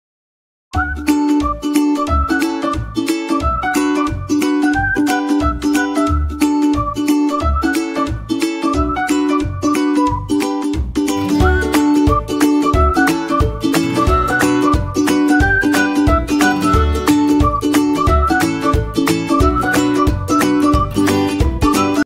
story enact by lkg
Cap seller and monkey story enact done by LKG kids